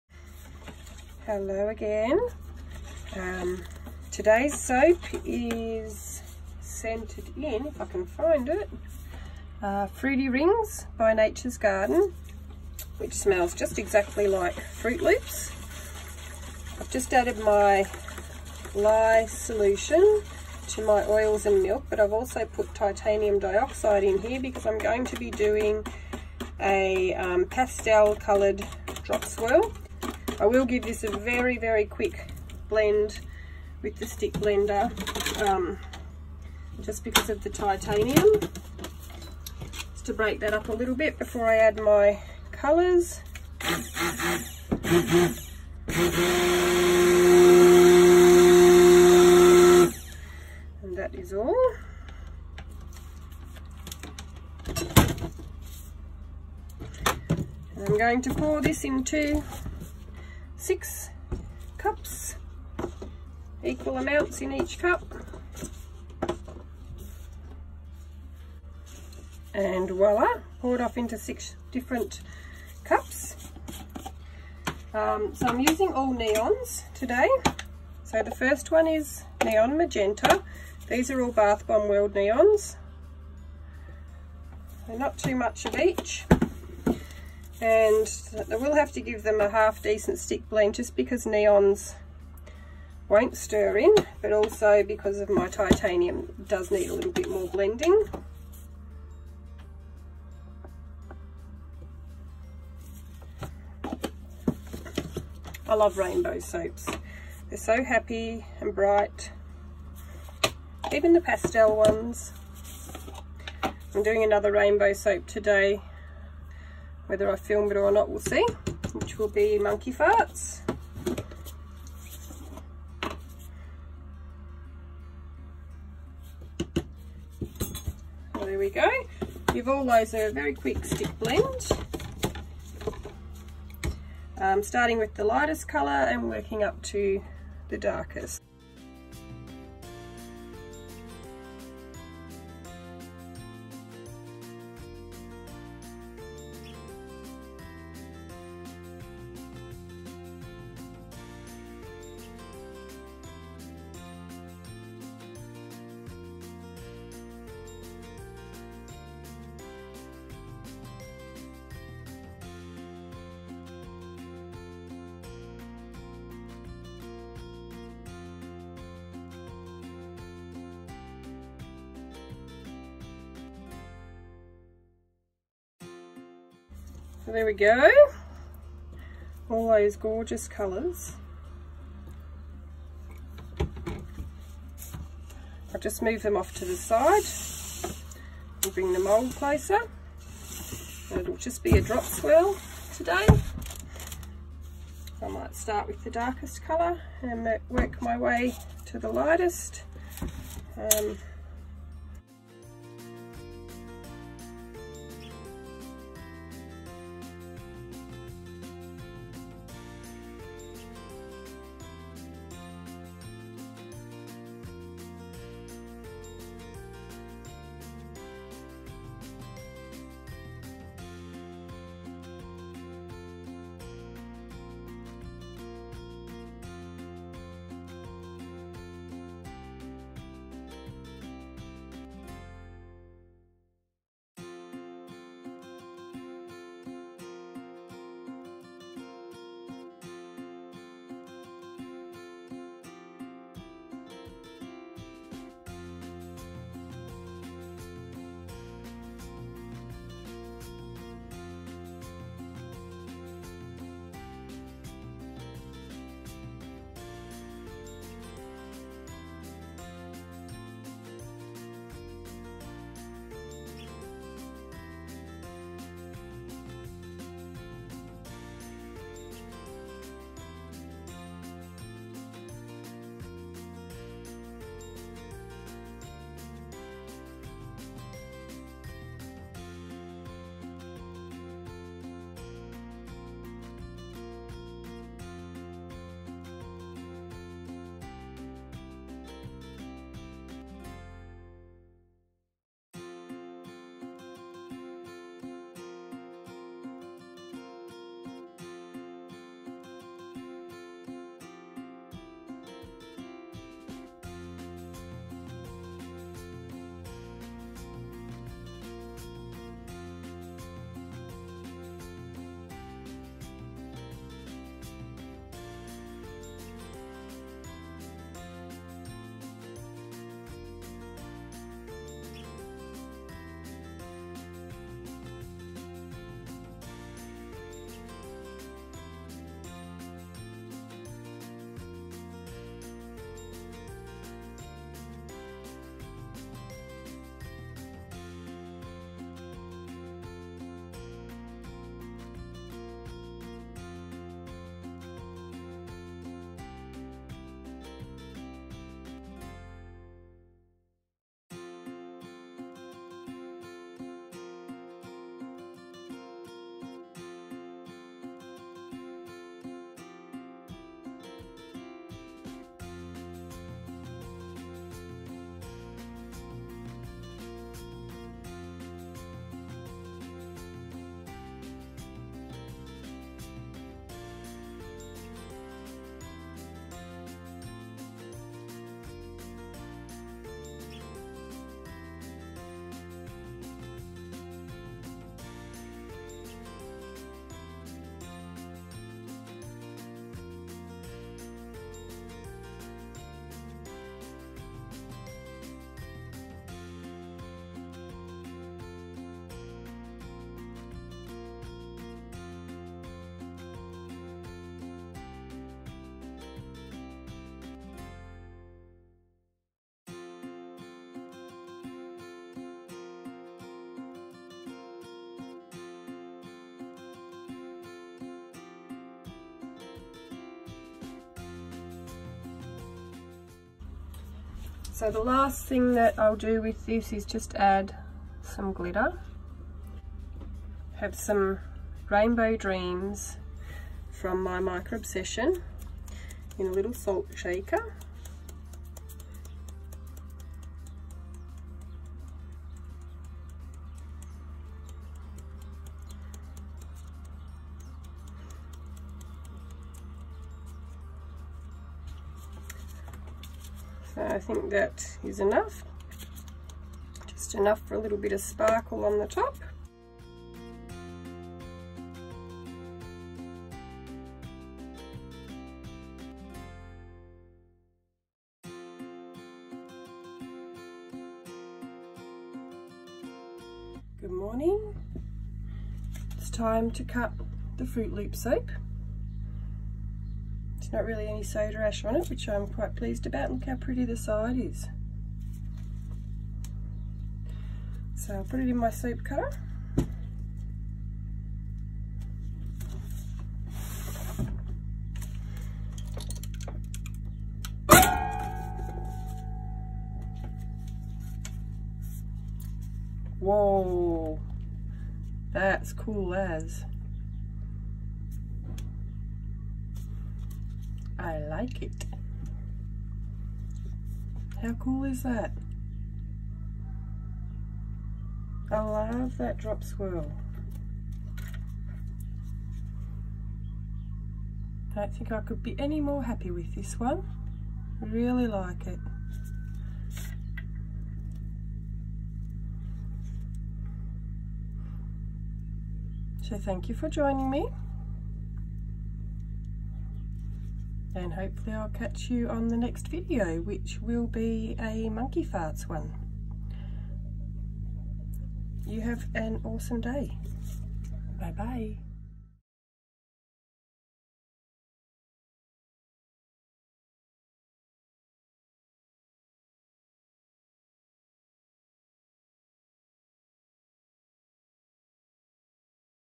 Froot Loops 🌈
Making and cutting of Froot Loops soap, drop swirl technique